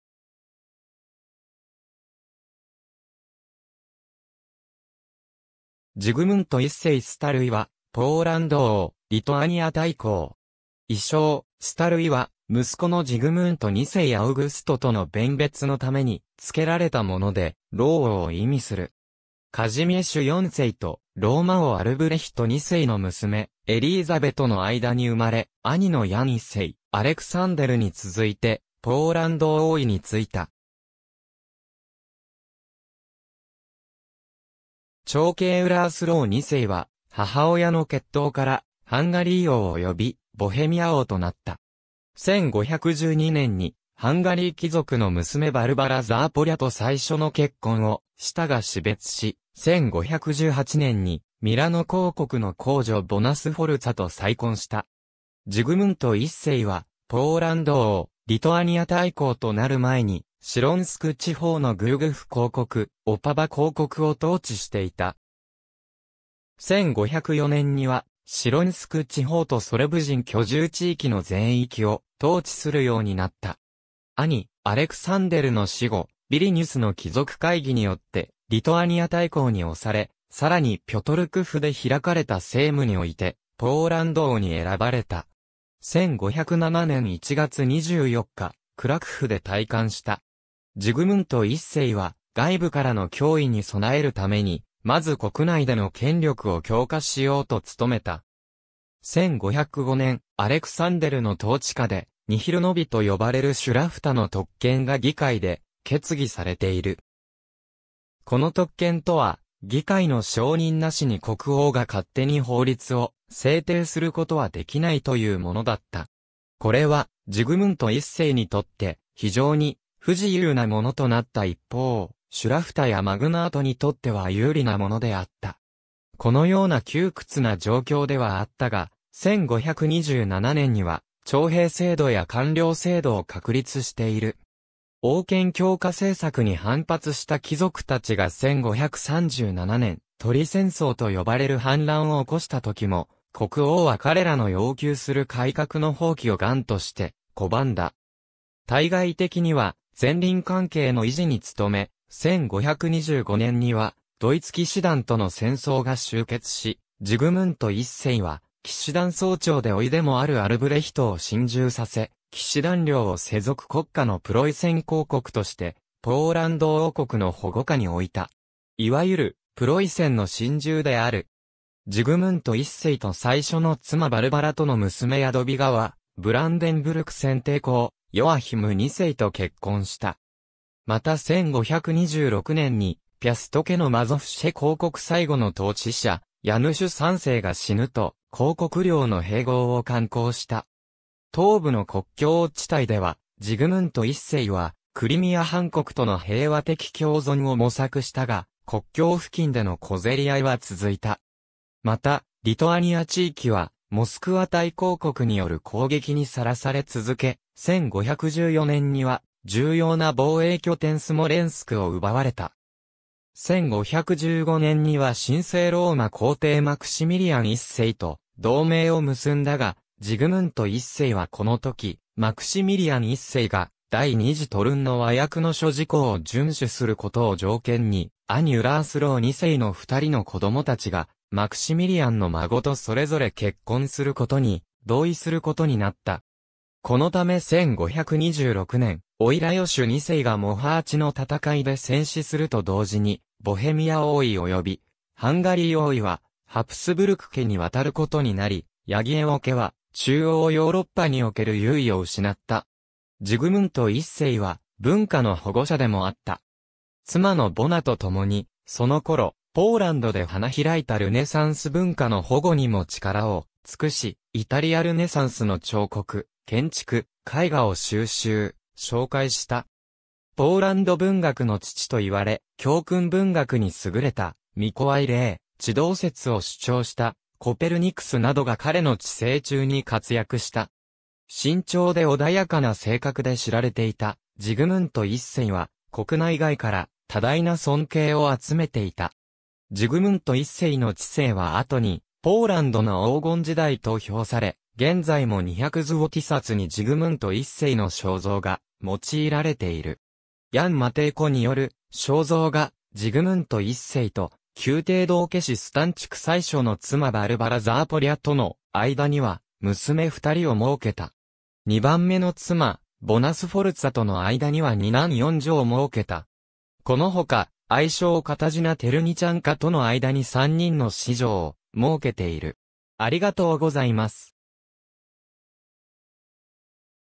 ジグムント1世 (ポーランド王)
ポーランド・ズウォティ紙幣の人物
ジグムント1世スタルィ（Zygmunt I Stary, 1467年1月1日 - 1548年4月1日）は、ポーランド王、リトアニア大公（在位：1506年 - 1548年）。
異称「スタルィ」（Stary）は息子のジグムント2世アウグストとの弁別のために付けられたもので、「老王」を意味する。
カジミェシュ4世と、ローマ王アルブレヒト2世の娘エリーザベトの間に生まれ、兄のヤン1世、アレクサンデルに続いてポーランド王位に就いた。
長兄ウラースロー2世は母親の血統からハンガリー王及びボヘミア王となった。
1512年にハンガリー貴族の娘バルバラ・ザーポリャと最初の結婚をしたが死別し、1518年にミラノ公国の公女ボナ・スフォルツァと再婚した。
ジグムント1世はポーランド王、リトアニア大公となる前に、シロンスク地方のグウォグフ公国（1499年 - 1506年）、オパヴァ公国（1501年 - 1506年）を統治していた。
1504年にはシロンスク地方とソルブ人居住地域の全域を統治するようになった。
兄アレクサンデルの死後、ヴィリニュスの貴族会議によってリトアニア大公に推され（1506年10月20日）、さらにピョトルクフで開かれたセイム（議会）においてポーランド王に選ばれた（1506年12月8日）。
1507年1月24日、クラクフで戴冠した。
ジグムント1世は外部からの脅威に備えるために、まず国内での権力を強化しようと努めた。
1505年、アレクサンデルの統治下で「ニヒル･ノヴィ」と呼ばれるシュラフタの特権が議会で決議されている。
この特権とは、議会の承認なしに国王が勝手に法律を制定することはできないというものだった。
これはジグムント1世にとって非常に不自由なものとなった一方、シュラフタやマグナート（大貴族）にとっては有利なものであった。
このような窮屈な状況ではあったが、1527年には徴兵制度や官僚制度を確立している。
王権強化政策に反発した貴族たちが1537年、鶏戦争と呼ばれる反乱を起こした時も、国王は彼らの要求する改革の放棄を頑として拒んだ。
対外的には善隣関係の維持に努め、1525年にはドイツ騎士団との戦争が終結し、ジグムント1世は騎士団総長で甥でもあるアルブレヒトを臣従させ、騎士団領を世俗国家のプロイセン公国としてポーランド王国の保護下に置いた。
いわゆる「プロイセンの臣従」である。
ジグムント1世と最初の妻バルバラとの娘ヤドヴィガは、ブランデンブルク選帝侯ヨアヒム2世と結婚した。
また1526年にピャスト家のマゾフシェ公国最後の統治者ヤヌシュ3世が死ぬと、公国領の併合を敢行した。
東部の国境地帯では、ジグムント1世はクリミア・ハン国との平和的共存を模索したが、国境付近での小競り合いは続いた。
またリトアニア地域はモスクワ大公国による攻撃にさらされ続け、1514年には重要な防衛拠点スモレンスクを奪われた。
1515年には神聖ローマ皇帝マクシミリアン1世と同盟を結んだが、ジグムント1世はこの時、マクシミリアン1世が第二次トルンの和約の諸事項を遵守することを条件に、兄ウラースロー2世の2人の子供達がマクシミリアンの孫とそれぞれ結婚することに同意することになった（ウィーン二重結婚）。
このため1526年、甥ラヨシュ2世がモハーチの戦いで戦死すると同時に、ボヘミア王位及びハンガリー王位はハプスブルク家に渡ることになり、ヤギェウォ家は中央ヨーロッパにおける優位を失った。
ジグムント1世は文化の保護者でもあった。
妻のボナと共に、そのころポーランドで花開いたルネサンス文化の保護にも力を尽くし、イタリア・ルネサンスの彫刻、建築、絵画を収集、紹介した。
ポーランド文学の父といわれ、教訓文学に優れたミコワイ・レイ、地動説を主張したコペルニクスなどがかれの治世中に活躍した。
慎重で穏やかな性格で知られていたジグムント1世は、国内外から多大な尊敬を集めていた。
ジグムント1世の治世は後に「ポーランドの黄金時代」と評され、現在も200ズウォティ札にジグムント1世の肖像が用いられている。
ヤン・マテイコによる肖像画 ジグムント1世と宮廷道化師スタンチク 最初の妻バルバラ・ザーポリャとの間には、娘2人をもうけた。
2番目の妻ボナ・スフォルツァとの間には2男4女をもうけた。
この他、愛妾カタジナ・テルニチャンカとの間...